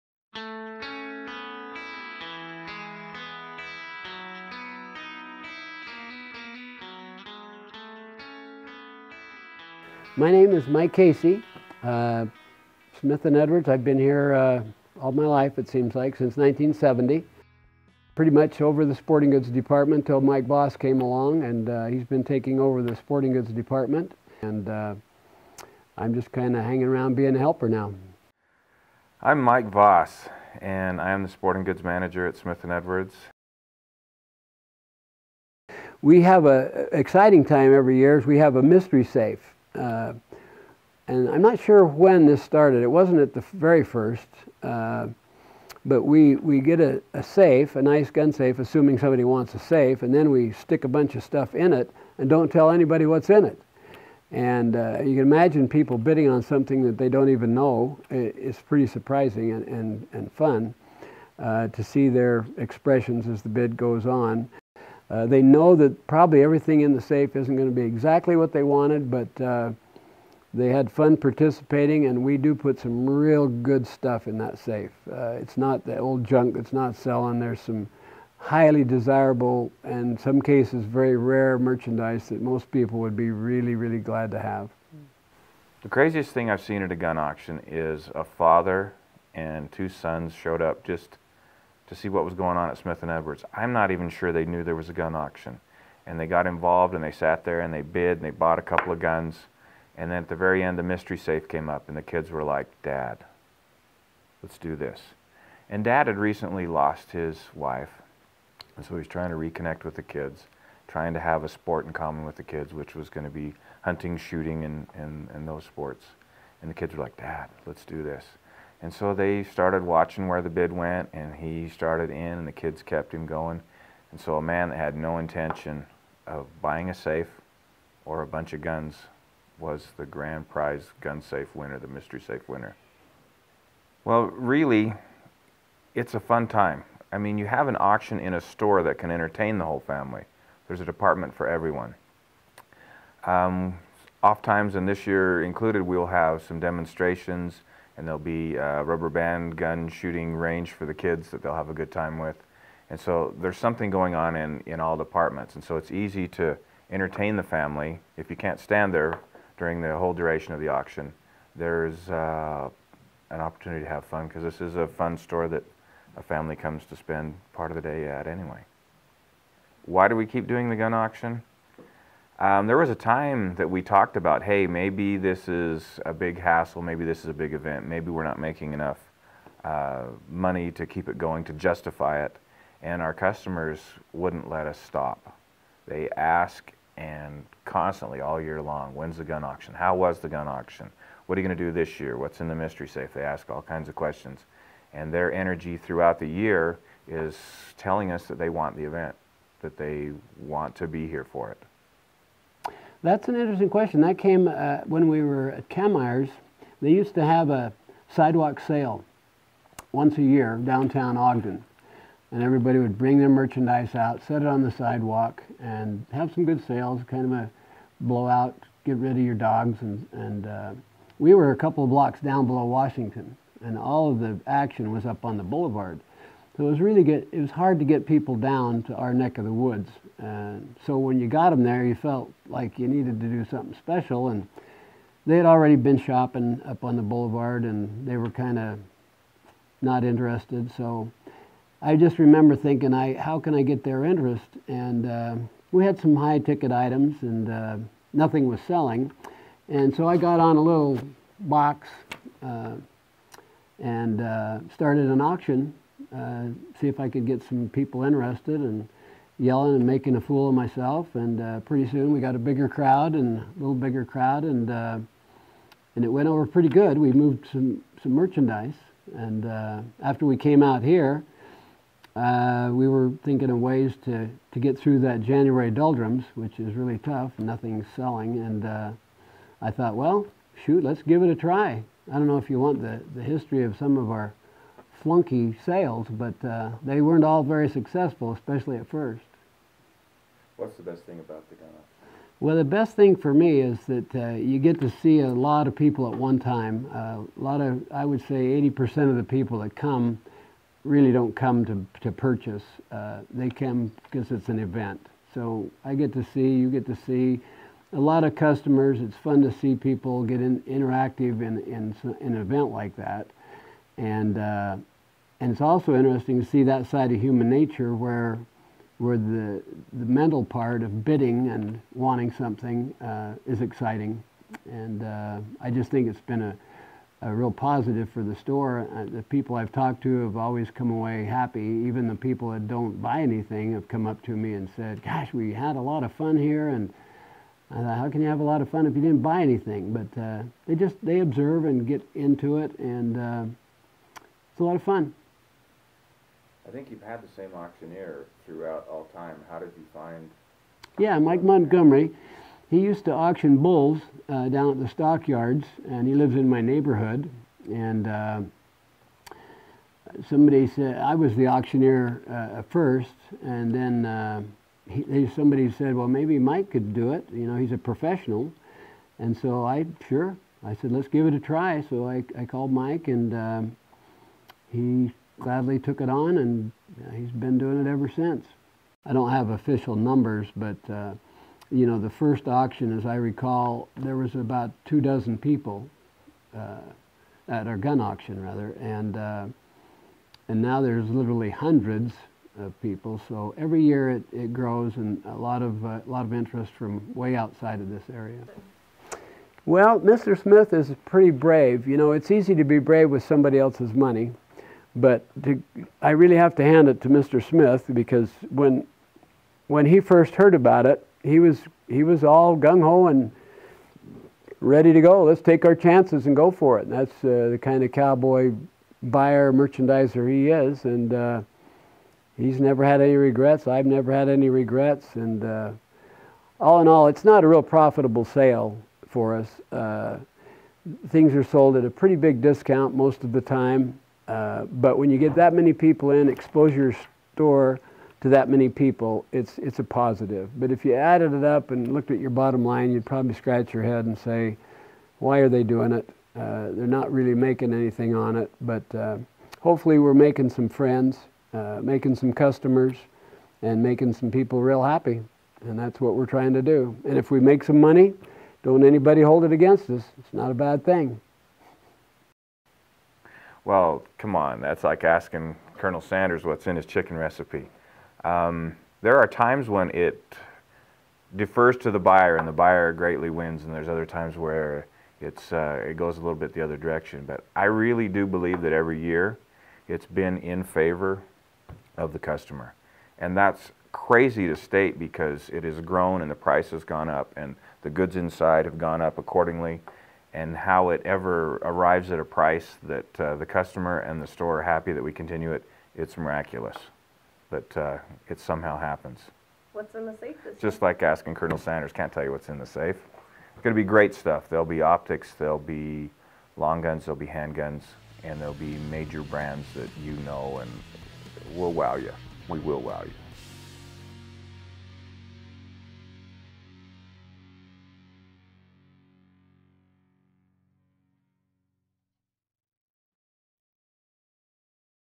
How the Gun Auction started...
Smith & Edwards' Gun Auction has been a tradition for 32 years! Discover how Mike Casey came up with the idea and how the tradition will continue.